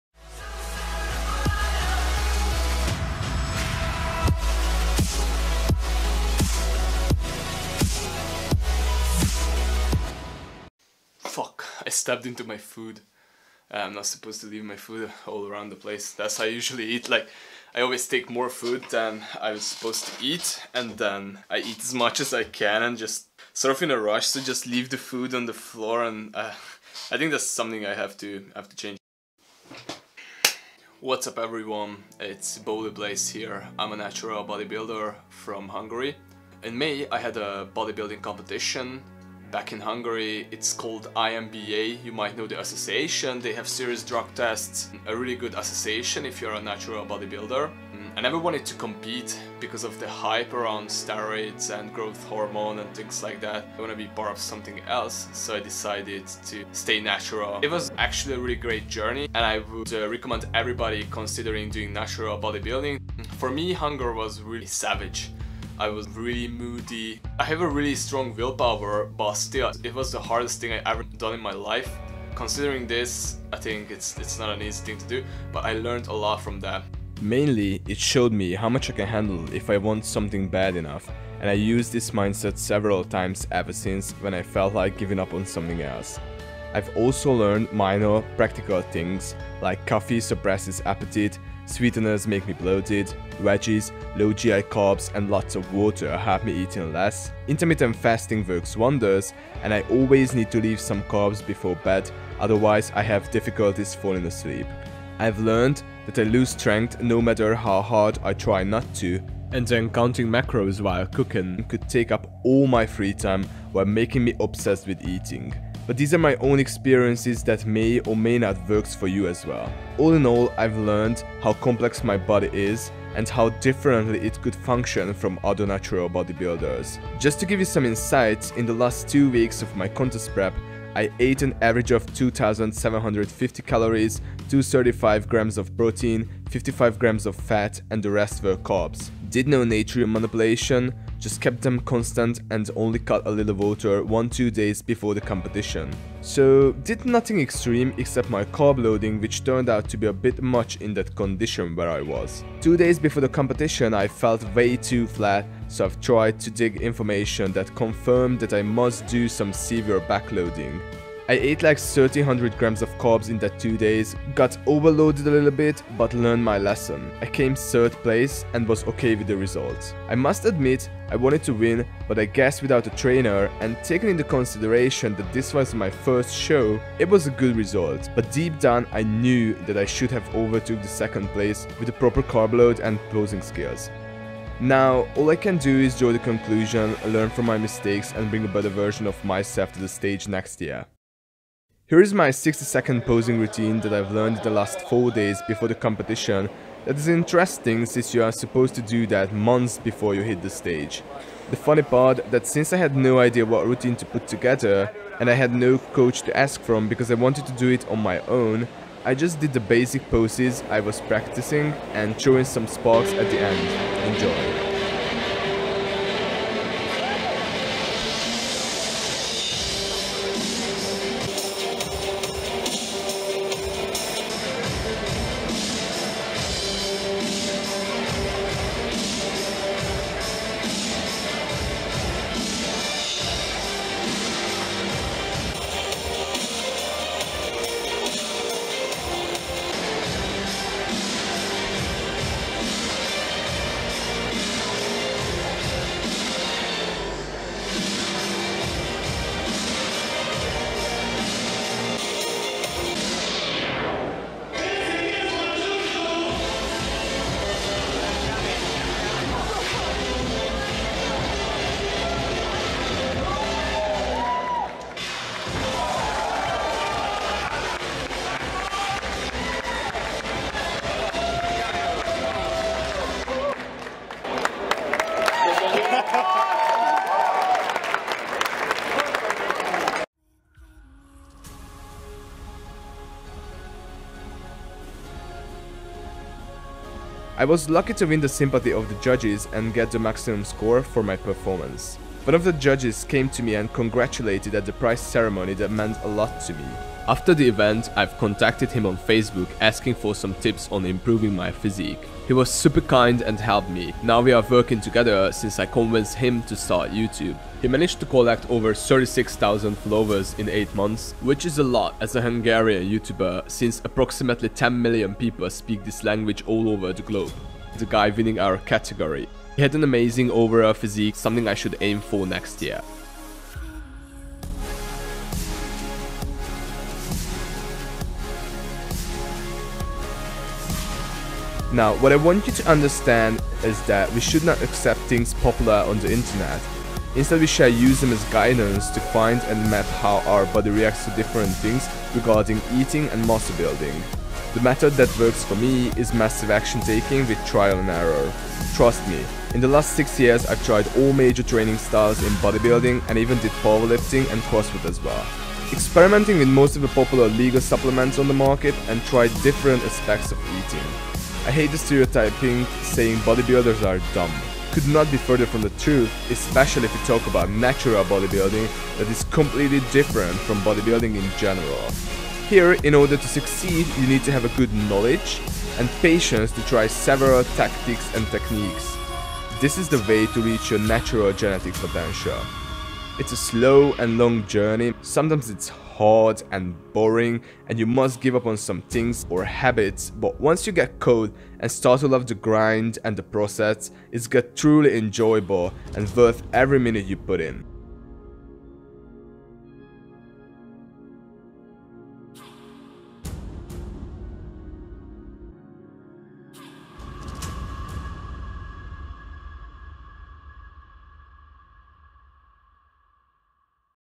🧐 Summary of my first natural bodybuilding competition 💪🏼
My name is Bowly Blaise and I’m a natural bodybuilder from Hungary. In May I had a natural bodybuilding competition back in Hungary. It’s called INBA. Perfect association if you’re a natural bodybuilder since they have strict drug testing policy. I’ve never wanted to compete because of the hype around steroids and growth hormone and things like that. I wanted to be part of something else, so I’ve decided to stay natural. It is a great journey and I’d recommend to everyone. To me, hunger was the toughest thing to deal with, but I’ve learned a lot from that. Mainly it showed me how much I can handle if I want something bad enough and I’ve used this mindset several times ever since when I felt like giving up on something else. I’ve also learned minor practical things like coffee suppresses appetite, sweeteners make me bloated, veggies, low GI carbs and lots of water help me eating less, intermittent fasting works wonders, and I always need to leave some carbs before bed otherwise I have difficulties falling asleep. I’ve learned that I lose strength no matter how hard I try not to and that counting macros while cooking could take up all my free time while making me obsessed with eating. But these are my own experiences that may or may not works for you as well. All in all, I’ve learned how complex my body is and how differently it could function from other natural bodybuilders. Just to give you some insights, in the last 2 weeks of my contest prep, I ate an average of 2750 calories, 235g protein 55g fat and the rest were carbs. Did no sodium manipulation, just kept them constant and only cut a little water 1-2 days before the competition. So, did nothing extreme, except my carb loading which turned out to be a bit much in that condition where I was. 2 days before the competition I felt way too flat, so I’ve started to dig information that confirmed that I must do some severe backloading. I ate like 1300g carbs in that 2 days, got overloaded a little bit but learned my lesson. I came 3rd place and was ok with the result. I must admit I wanted to win but I guess without a trainer and taking into consideration that this was my 1st show, it was a good result but deep down I knew that I should have overtaken the 2nd place with a proper carb load and posing skills. Now all I can do is to draw the conclusion learn from my mistakes and bring a better version of myself to the stage next year.

Now what I want you to understand is that we should not accept things popular on the internet. Instead, we shall use them as guidance to find and map how our body reacts to different things regarding eating and muscle building. The method that works for me is massive action taking with trial and error.
Trust me in the last 6 years I’ve tried all major training styles in bodybuilding and even did powerlifting and CrossFit as well. Experimented with most of the popular legal supplements on the market and tried different aspects of eating. Vegan is something I must try soon but I know it’s going to be a battle for me. I hate the stereotyping saying bodybuilders are dumb. Could not be further from the truth especially if we talk about natural bodybuilding that is completely different from bodybuilding in general. Here to succeed, you need to have good knowledge and patience to try several tactics and techniques. This is the way to reach your natural genetic potential. It’s a slow and long journey, sometimes it’s hard and boring and you must give up on some things or habits but once you get caught and start to love the grind and the process it gets truly enjoyable and worth every minute you put in.